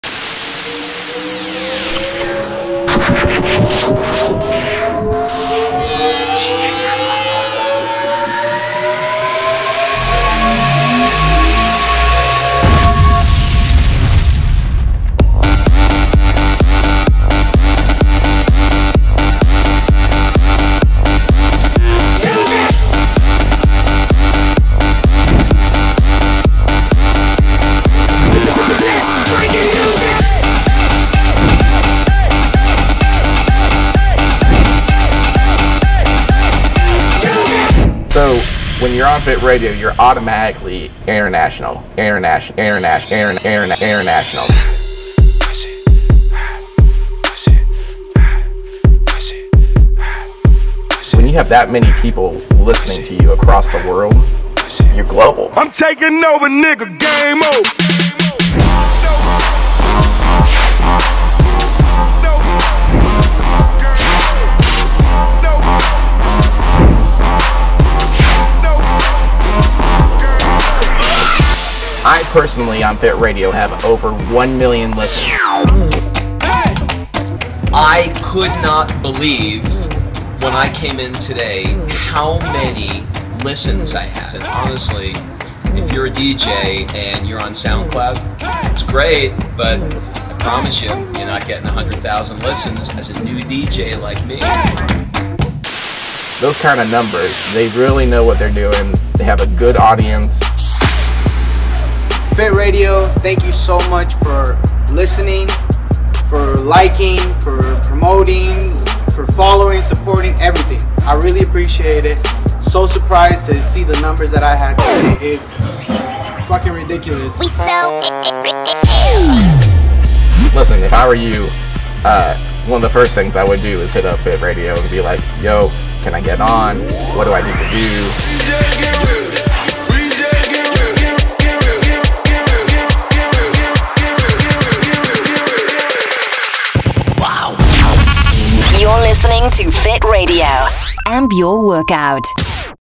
Become a FIT Radio DJ Now
As a FIT Radio DJ you go from being local to international. We have a built in audience you can NOT get from sound cloud or anywhere else.